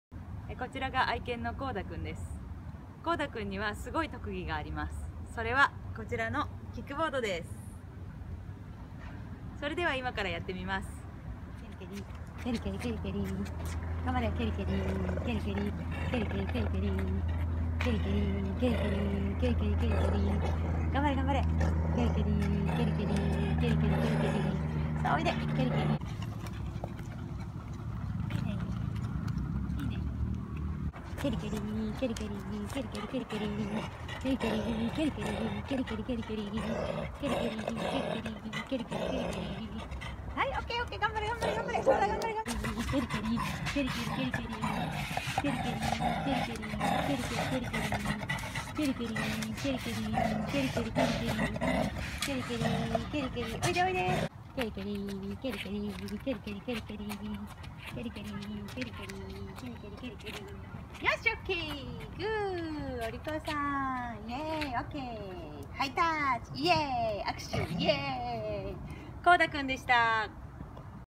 キックボード犬 コーダ4
スケボーとサーフィンができるので、もしかしたらと思ったら上手に乗りこなしちゃいました！